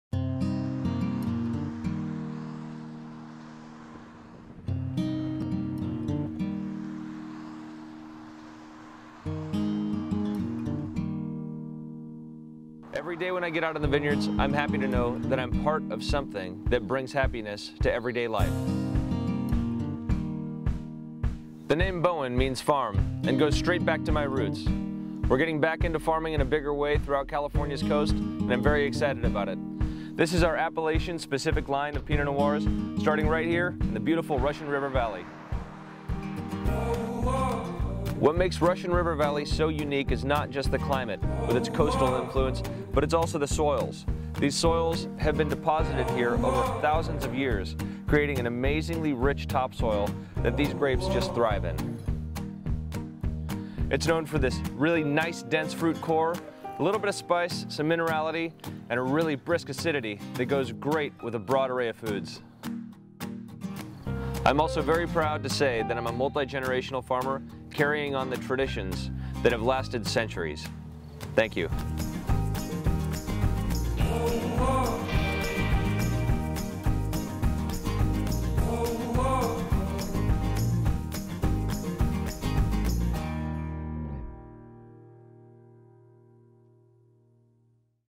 BÖEN Pinot Noir: Russian River Valley
A translation of ‘The Farm’, Böen is a reflection of Owner/Winemaker Joe Wagner’s agricultural roots as a lifelong grapegrower and winemaker. “I chose to create Böen as a reminder to myself that the wine I make is indivisible from the land it comes from, and that first and foremost, I am a farmer,” says Owner/Winemaker Joe Wagner. Böen joins a thriving portfolio of existing wine brands, including Belle Glos single vineyard Pinot Noirs from the coast of California and Elouan Pinot Noir from Oregon.

Boenwines.com